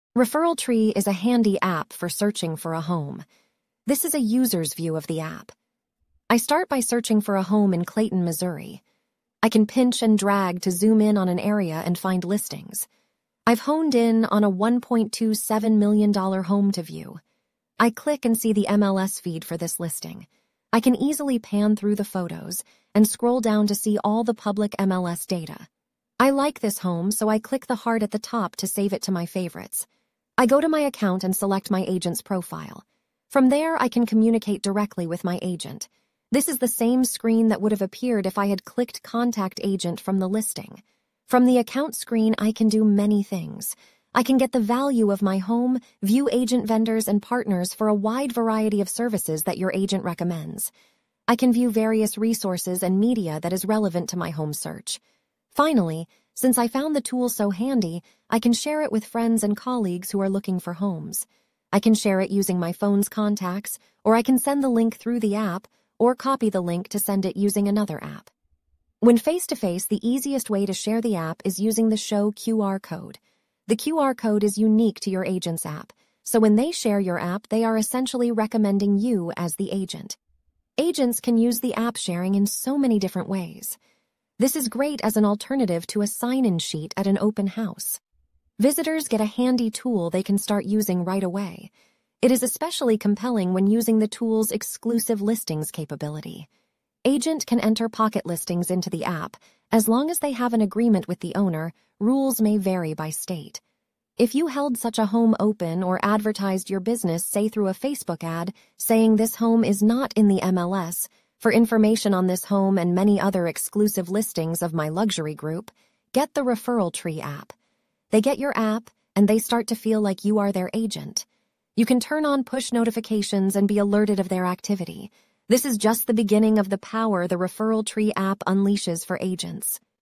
ref tree app walkthru
This is a demo walk-thru of my upcoming app. Please put the name of whoever told you about this in the sponsor field when you preregister.